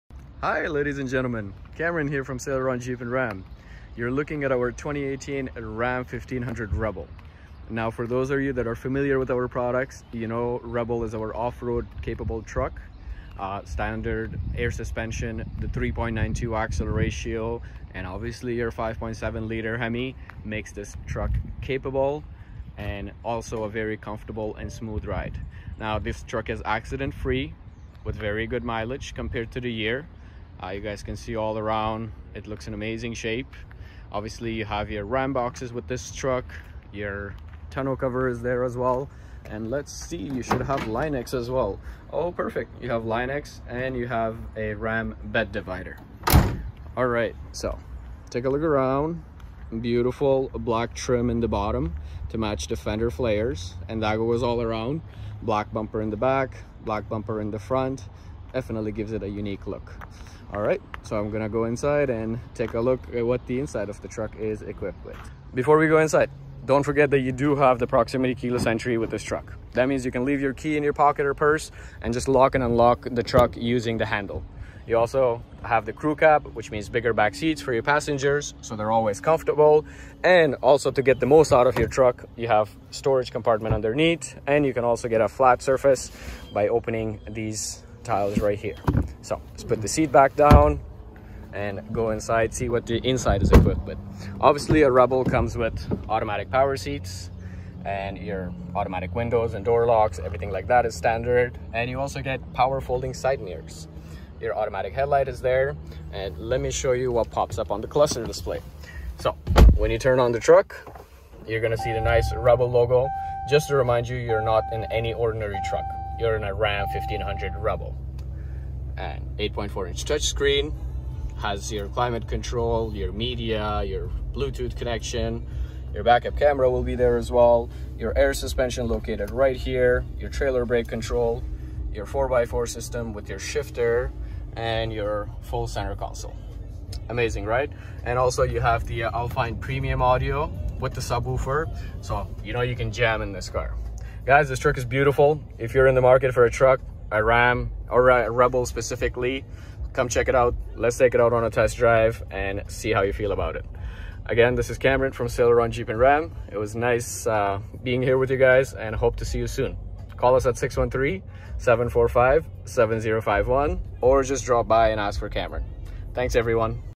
2018 RAM 1500 Rebel Crew Cab 4x4 | 900 St-Laurent Blvd, Ottawa, ON | Walkaround Review
2018 RAM 1500 Rebel Crew Cab 4x4 (Stock # 210276A) equipped with Alpine Audio, Factory Trailer Brake Controller, Ram Box, Rear Park Sense & more!

Body Style:   Pickup
Engine:   5.7L 8Cyl
Exterior:   Bright White
Interior:    Other
Fuel type:   Gasoline
Transmission:   Automatic
Drivetrain:   4x4
Stock #:   210276A

Ottawa St-Laurent Jeep & RAM | Top Rated Ottawa Dealership
900 St-Laurent Blvd, Ottawa, Ontario, K1K 3B3